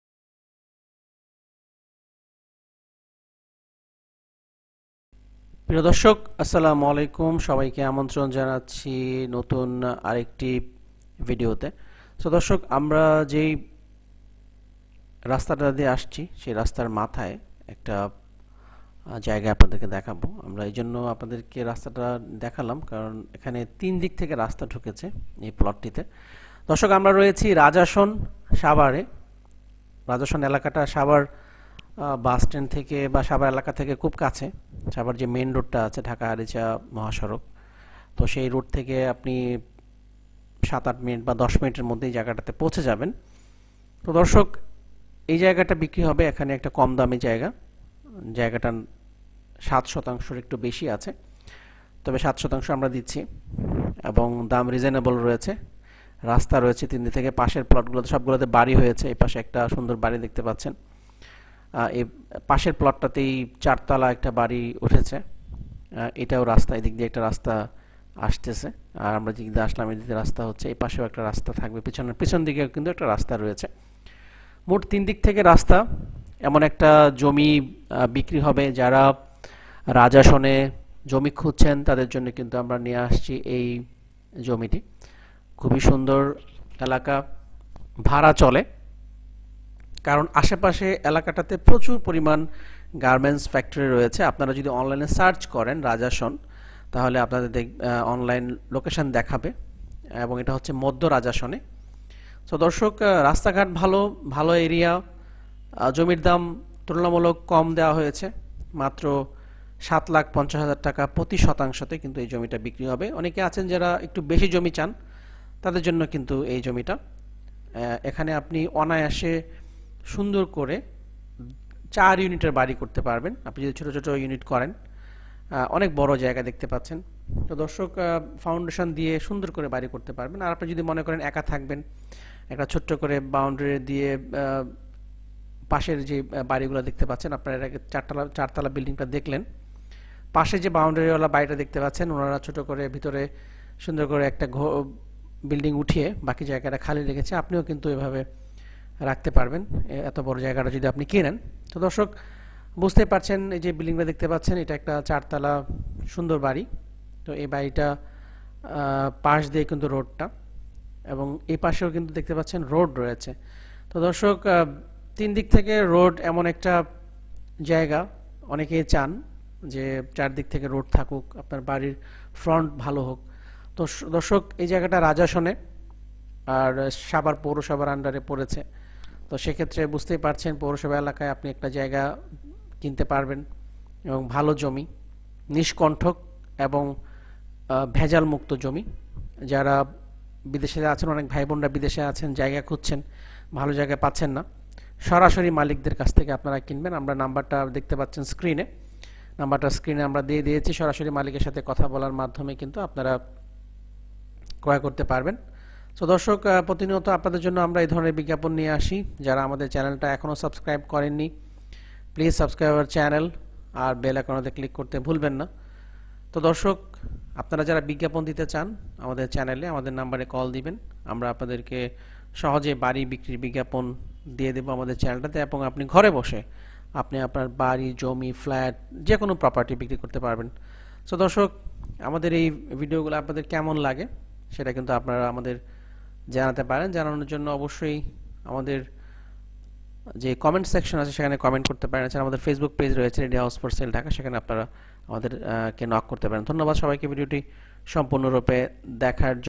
অনেক কমদামে বাড়ির জমি বিক্রয় হবে আর্জেন্ট ।। ready land sale in Rajashon savar Dhaka
৭ শতাংশ জমি বিক্রয় হবে যার মূল্য   ৭.৫ লাখ   টাকা  প্রতি শতাংশ
মালিক সরাসরি -০১৭১২-১৯৬৯৫১, ০১৬০৮-২২৭২৩১